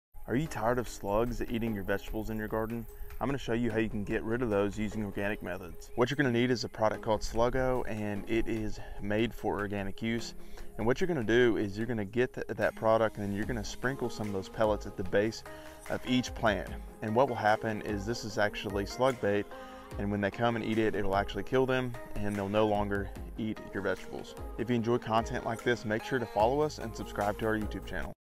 Here’s How to Control Slugs in Your Organic Garden
Wondering how to control slugs in your garden? Here is the best way to get rid of slugs in regarding using organic methods. We use a slug bait pellet called Sluggo and it works great

GARDEN SUPPLIES I USE

CHICKEN SUPPLIES

SEED STARTING SUPPLIES

BOOKS I RECOMMEND